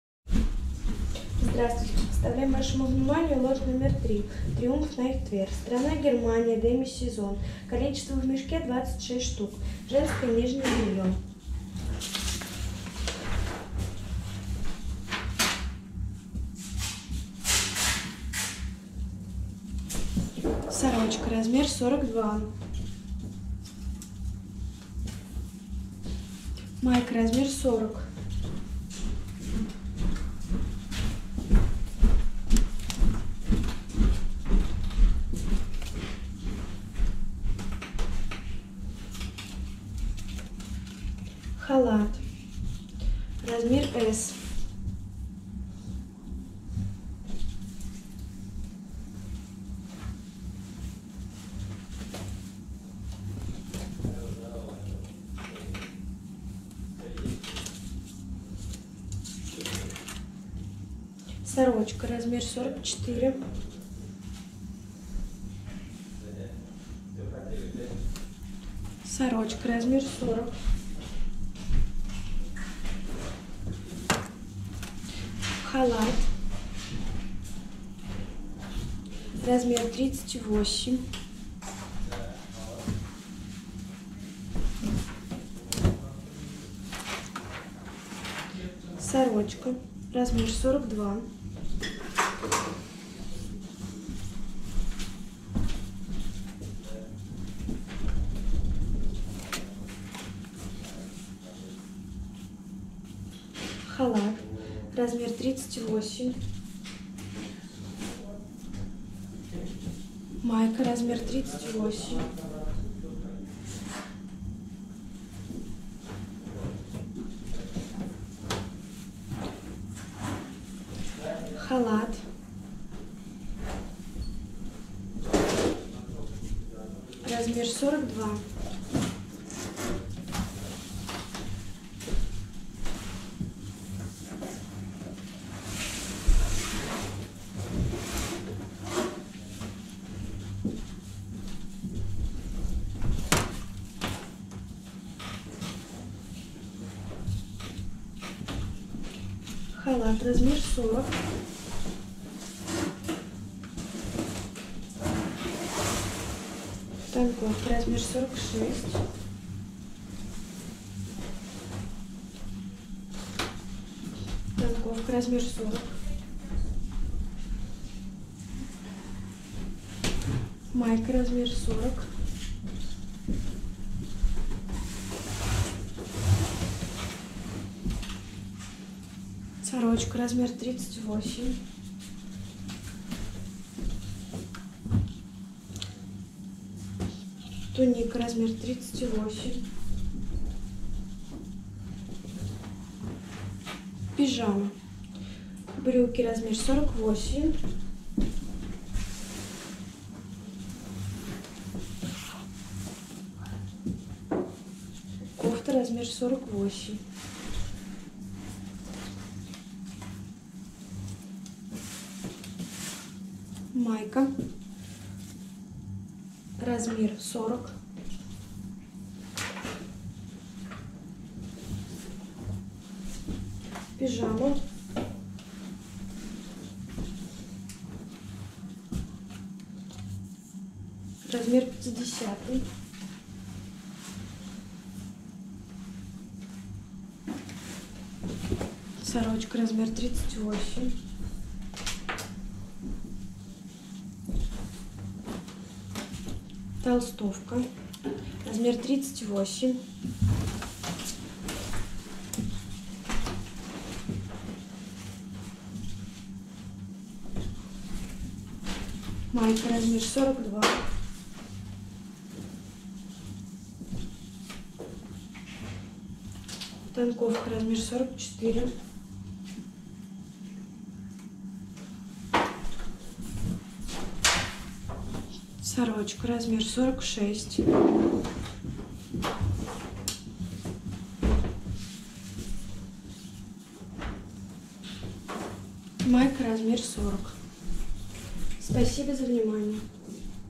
Triumf nightwear 3, сток одежда оптом
Triumph Nightwear (лот №3)
Женские ночные рубашки(сток)
Страна: Германия
Цена за ед.: 8.74 евро
Цена за мешок: 227.24 евро
Кол-во в мешке,шт.: 26